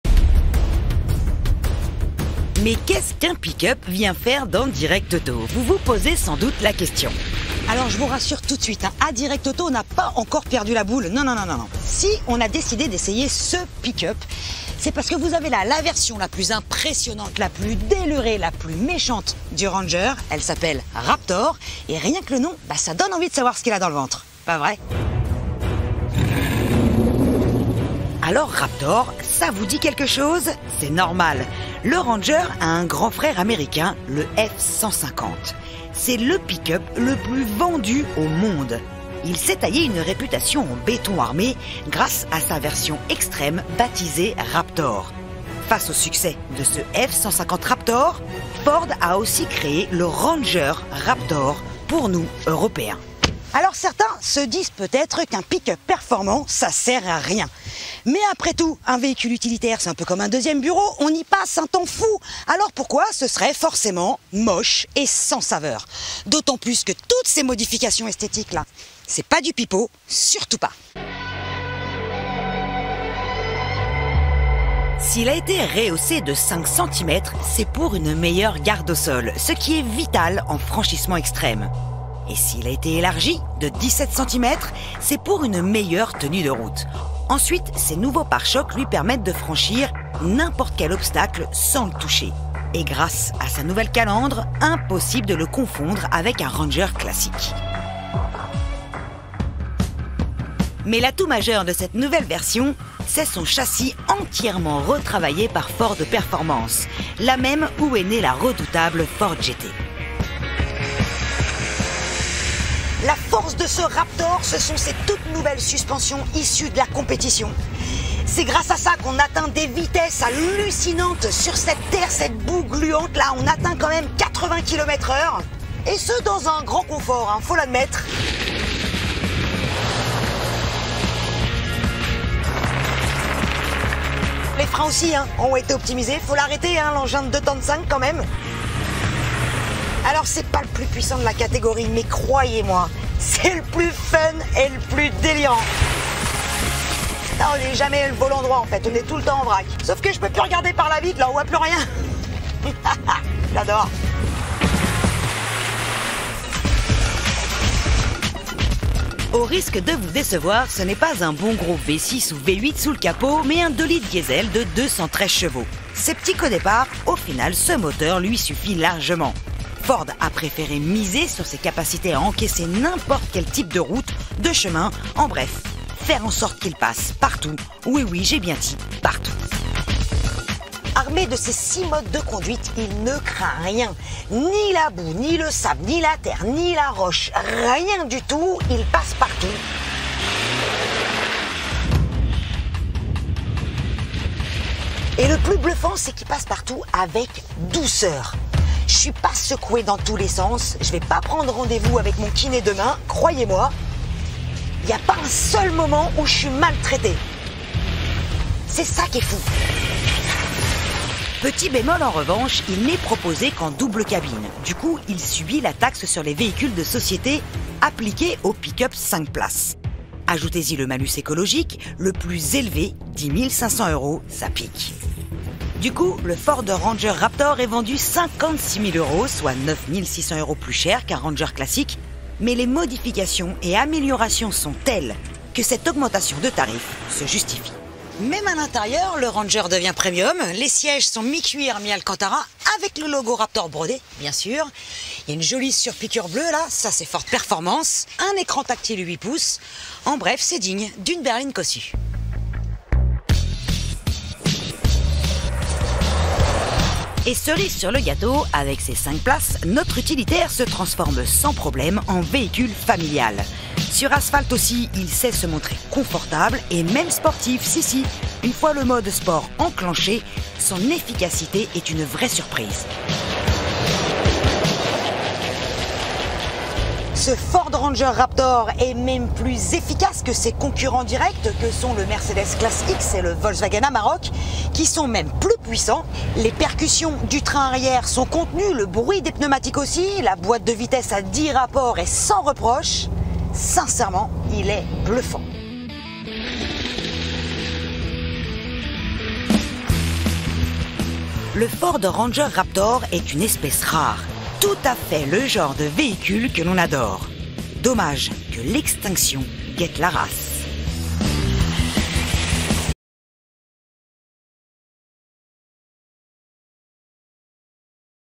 ESSAI : FORD RANGER RAPTOR
C’est le plus délirant des pickups disponible sur le marché français. Le Ford Ranger Raptor est le candidat idéal pour s’éclater hors des sentiers battus !